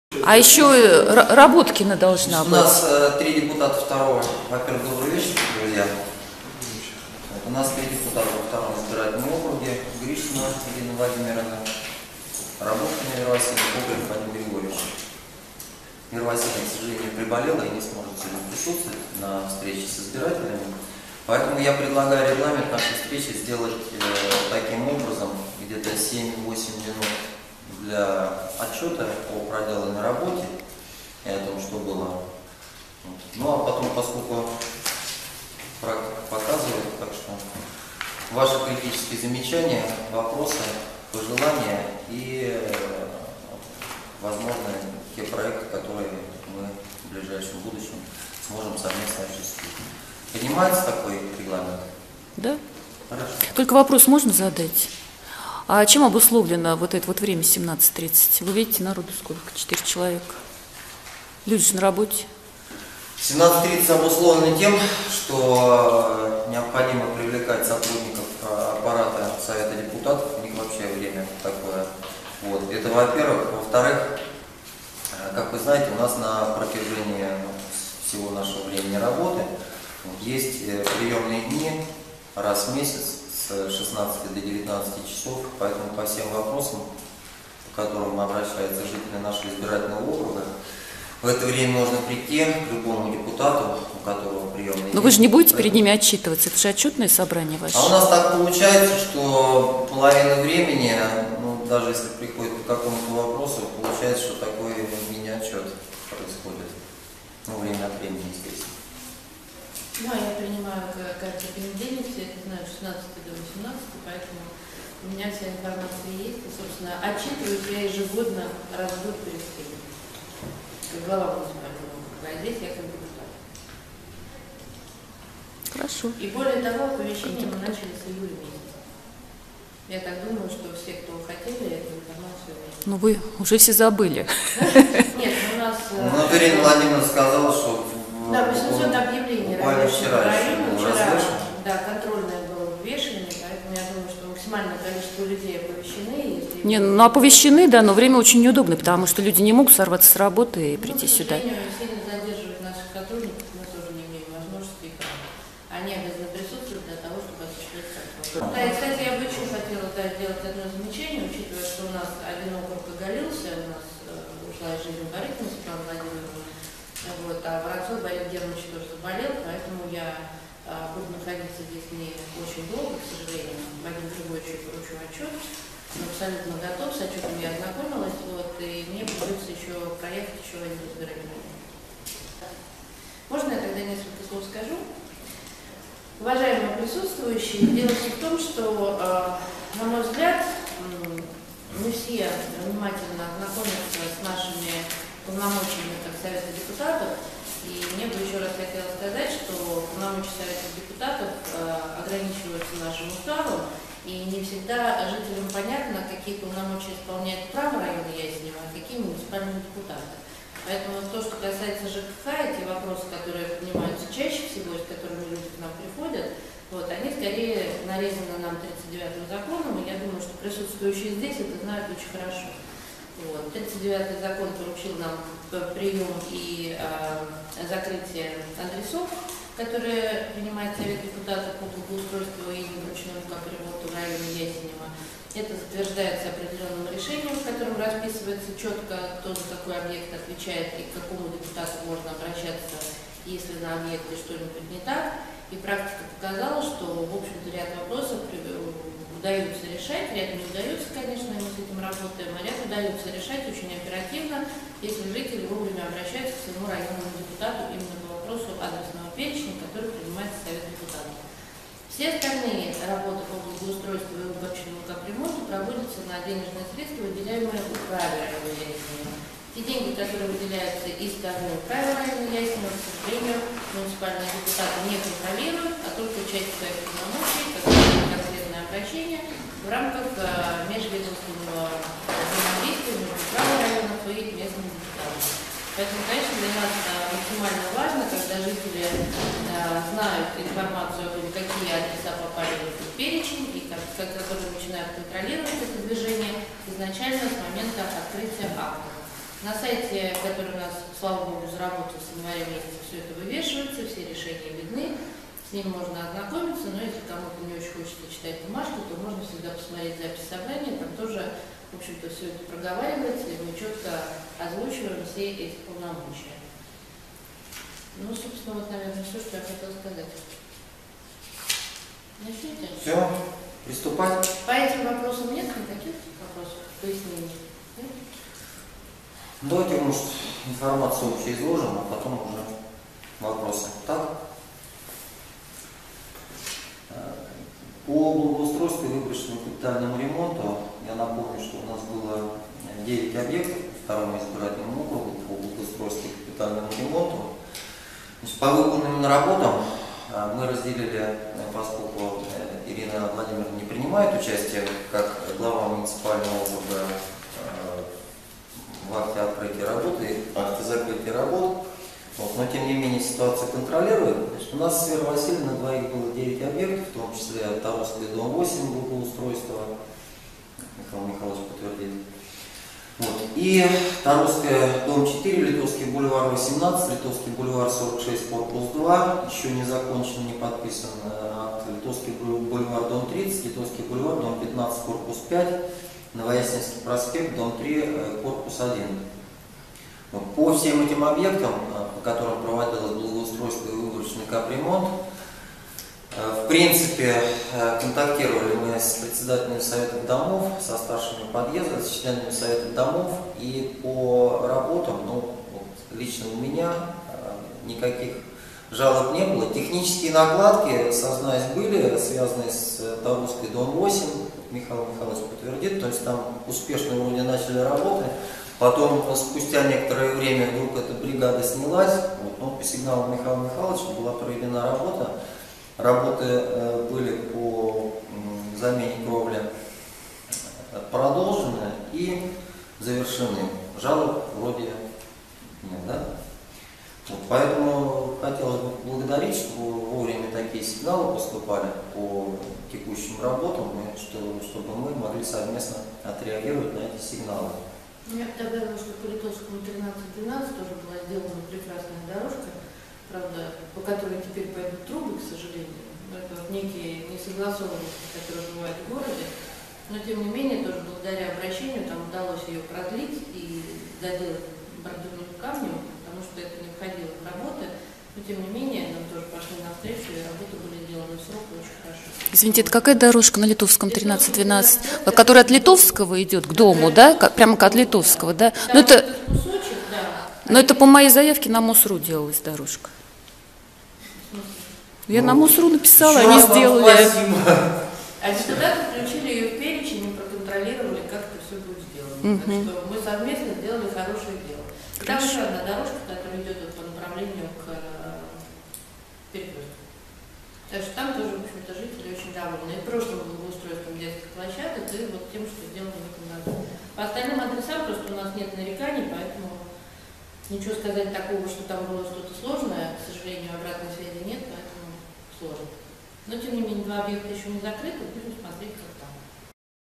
Часть 1. Отчет. Вступление.
18 сентября 2014 года. Отчет депутатов 2-го избирательного округа в школе 780.